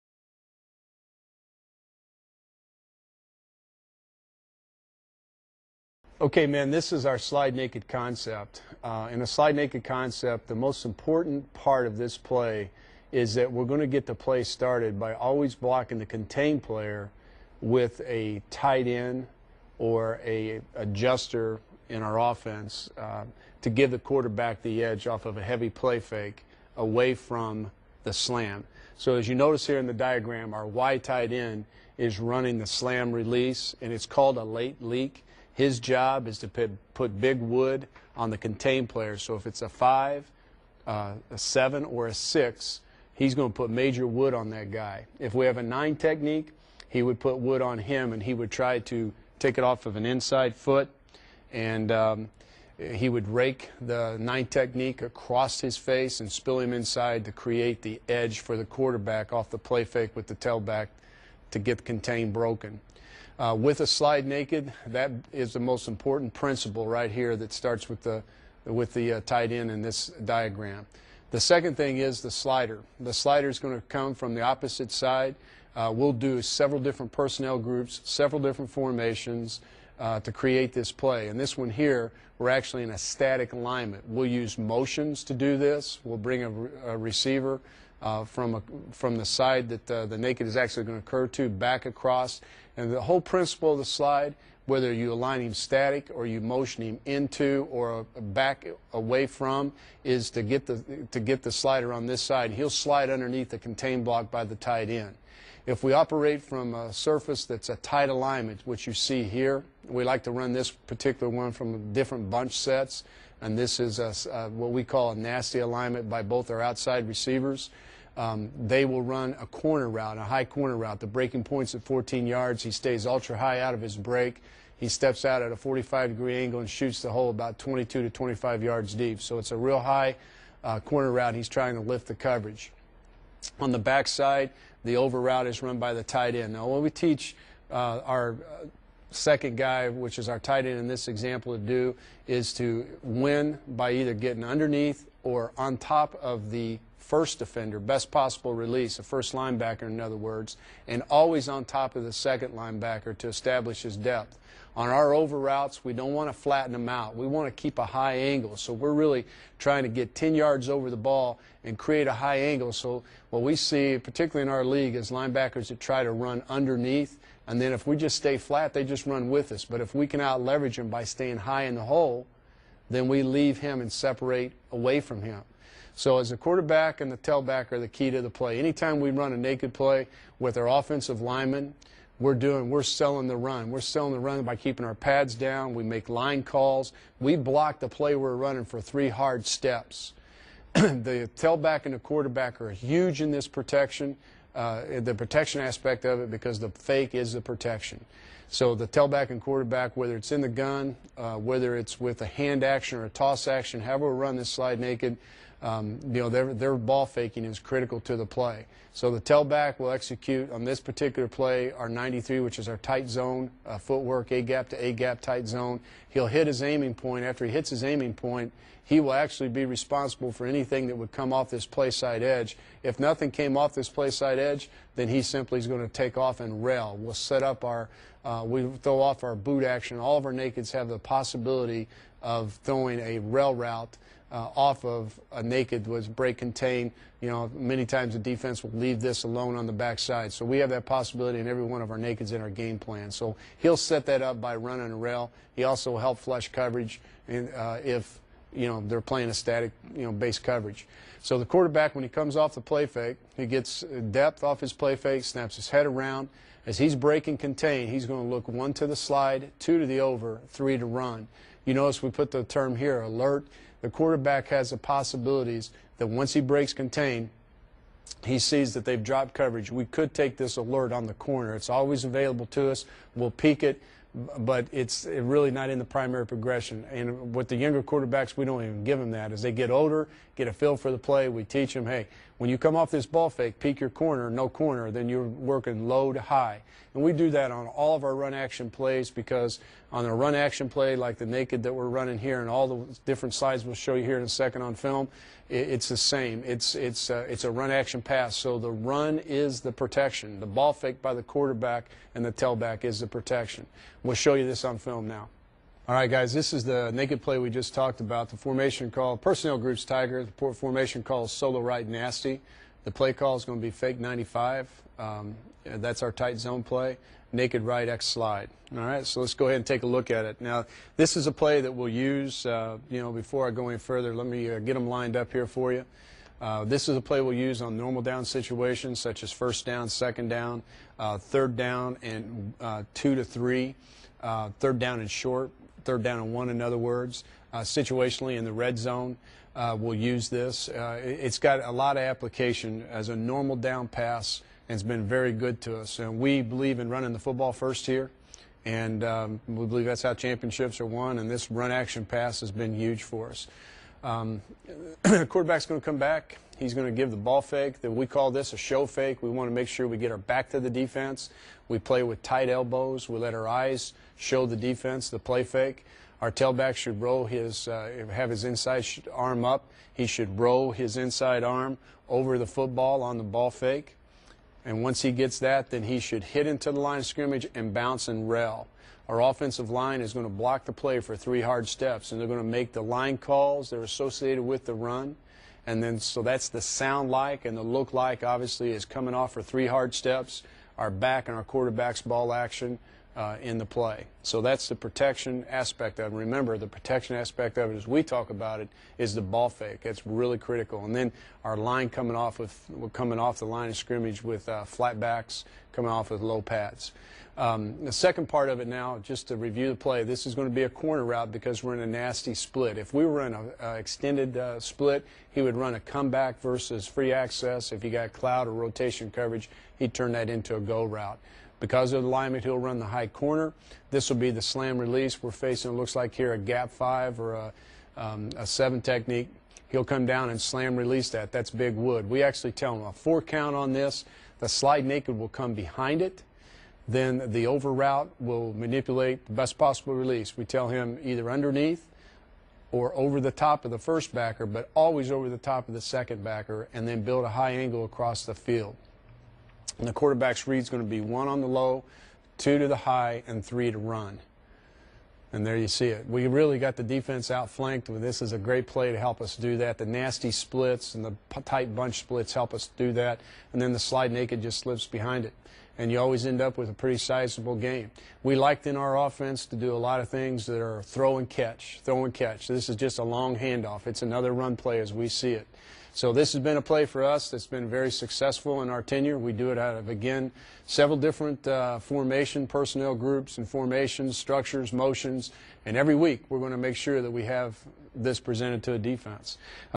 The Slide Naked Concept - From the Outside Zone & multiple Formations
If you run a form of the outside zone, take a look at this Play Action concept. Utilize it from multiple personnel situations and multiple formations.